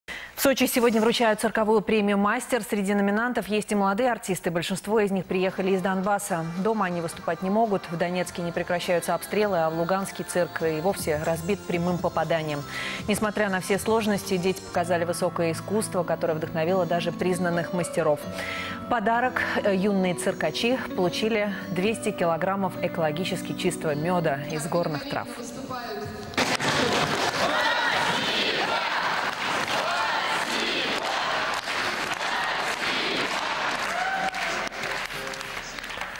В Сочи вручают цирковых «Оскаров»
На получение профессиональной награды было подано больше 400 заявок из разных стран мира. В том числе - из России, Белоруссии,Австралии, Китая, Италии, Мексики, Украины, Испании, Новой Зеландии, Швейцарии. По всем работам проводилось голосование в Интернете. В результате определились по пять претендентов в каждой из номинаций. Обладатели цирковых «Оскаров» будут названы в 12 номинациях: «Аттракцион года», «Номер года», «Цирковое шоу года», «Артист года», «Артистка года», «Режиссер года», «Художник года», «Дрессировщик года», «Клоун года», «Композитор года», «Хореограф года» и «Продюсер года».Международную премию «Мастер», которую чаще называют цирковым «Оскаром», вручают в Сочи. Торжественная церемония вручения престижных наград пройдет вечером 30 июня в Сочинском цирке.